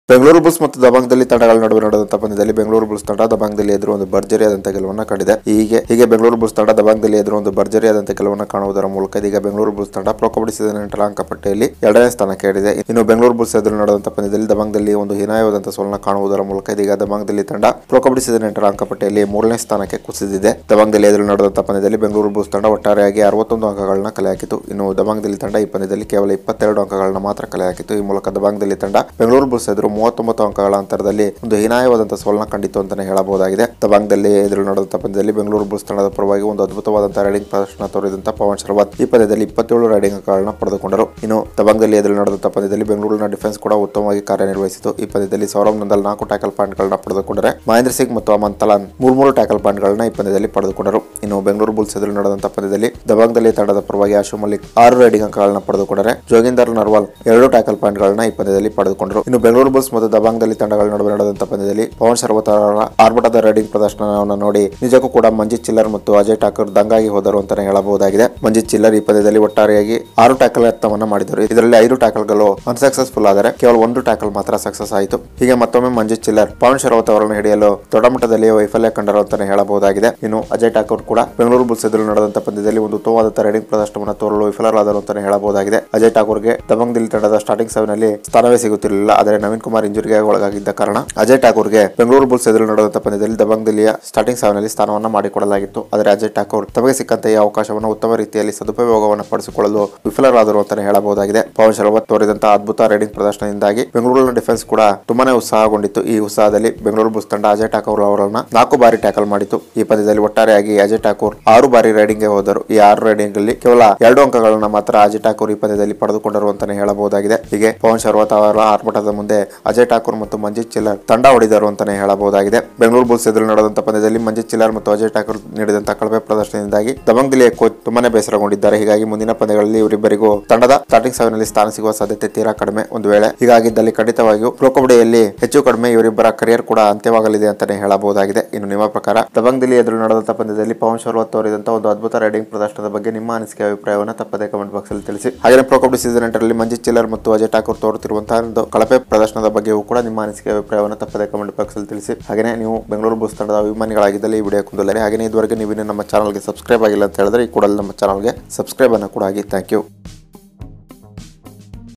ಮಂಜಿತ್ ಚಿಲ್ಲಾರ್ ಮತ್ತು ಅಜಯ್ ಠಾಕೂರ್ ಅವರ ಪ್ರೊ ಕಬ್ಬಡ್ಡಿ ಕೆರಿಯರ್ ಅಂತ್ಯ ಮಾಡಿದ ಪವನ್.!!
Take risks on your Own.